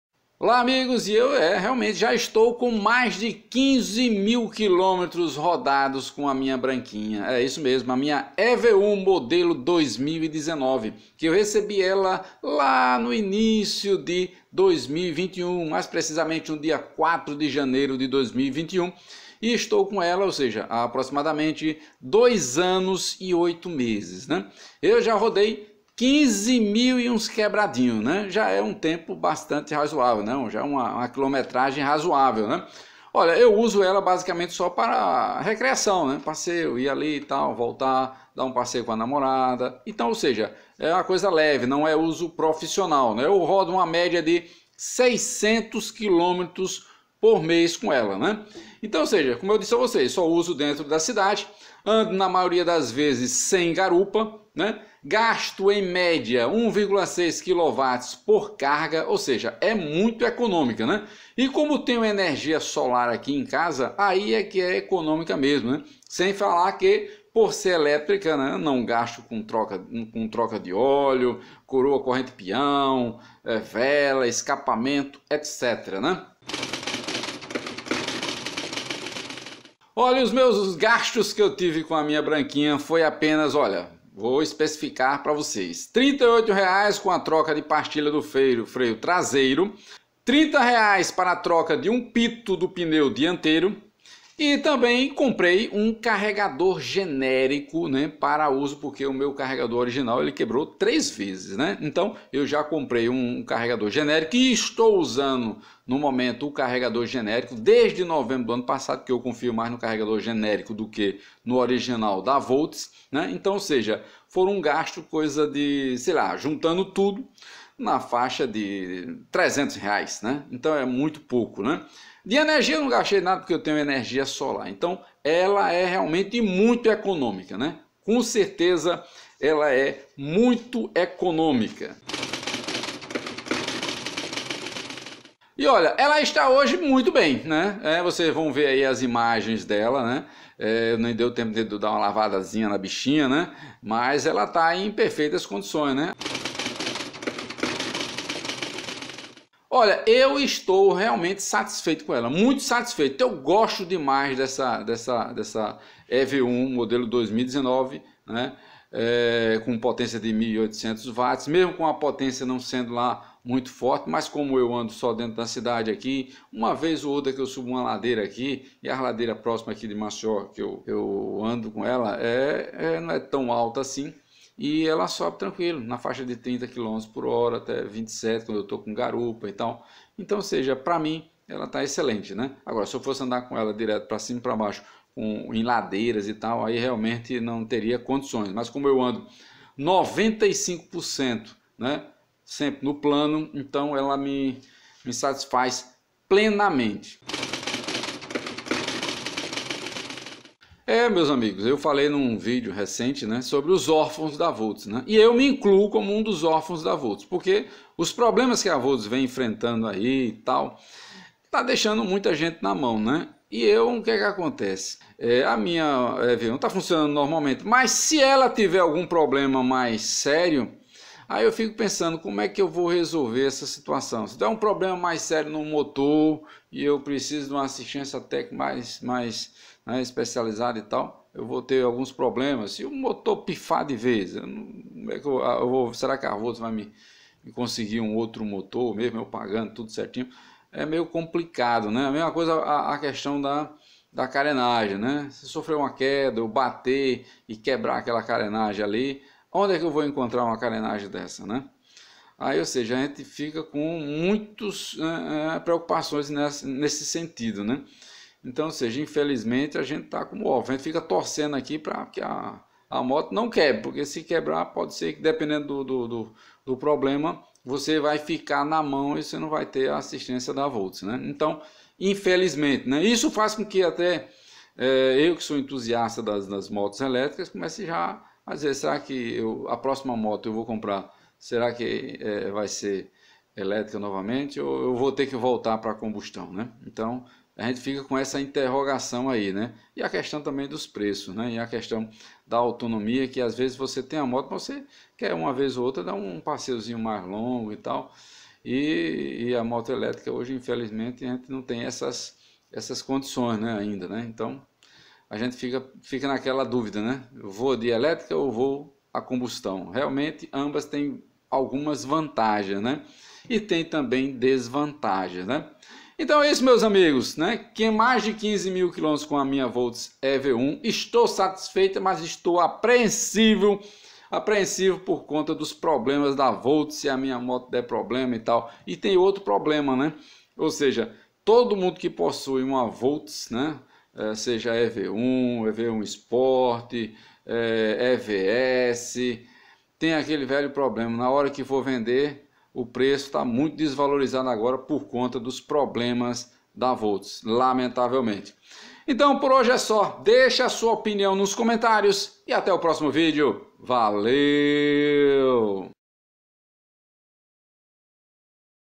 VOLTZ EV1 DEPOIS DE 15 MIL KM
Neste vídeo falarei sobre como está a minha scooter Voltz EV1 modelo 2019, depois de 15.000 KM rodados e as minhas preocupações com o futuro.